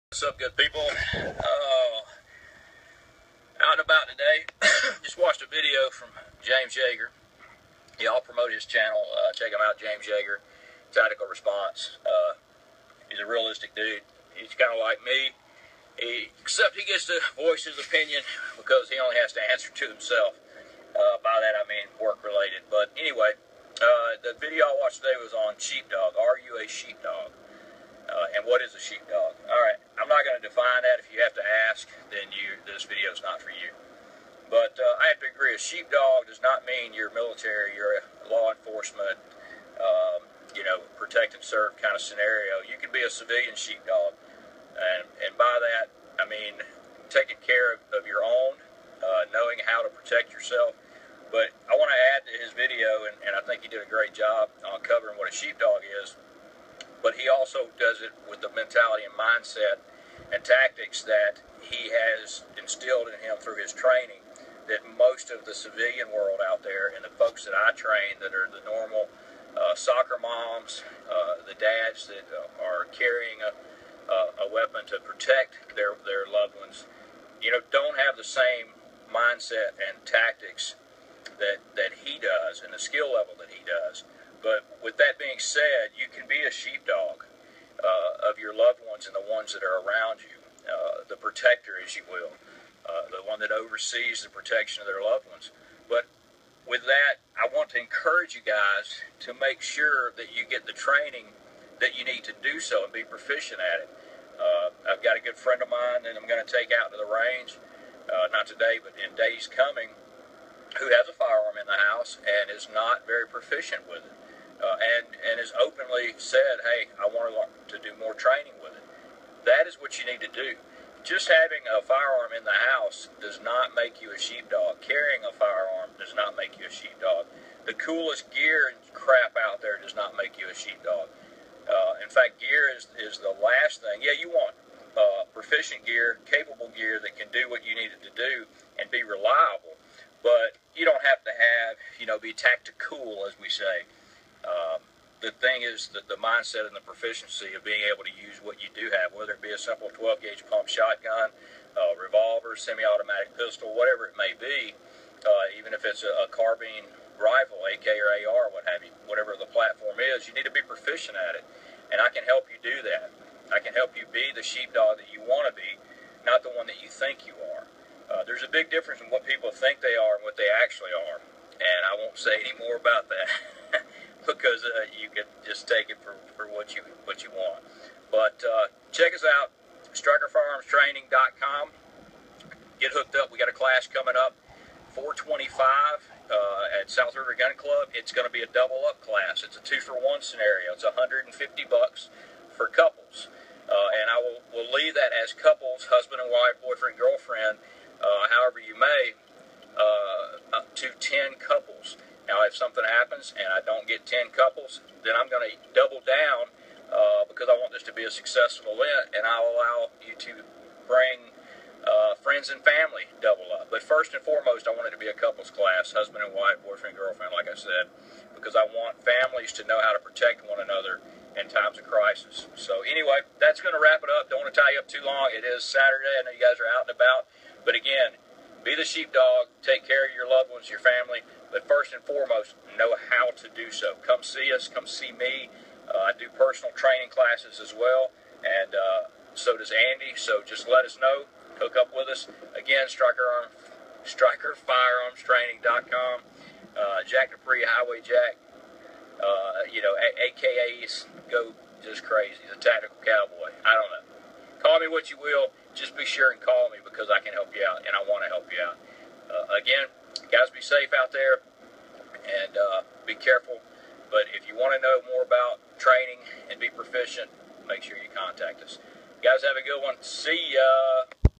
Concealed Carry & Home Protection Are you the sheepdog ?
Are you the sheepdog and protector of you family and loved ones? We are offering a one time couples class at a huge discount on 4/24/15 Sorry for the delay and lag between audio and video. Don't know what happened there.